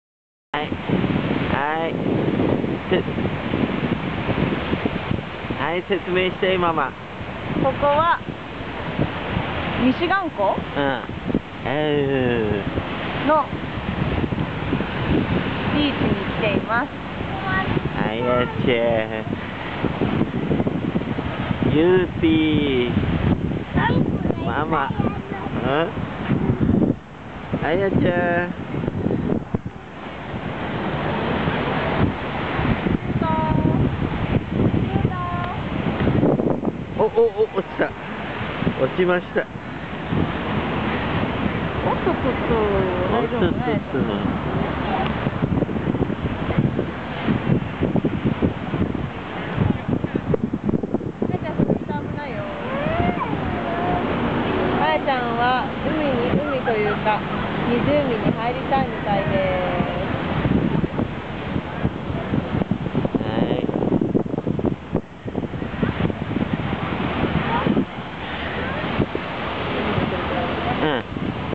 Yutie & Aya at Lake Michigan
Walking at beach of Lake Michigan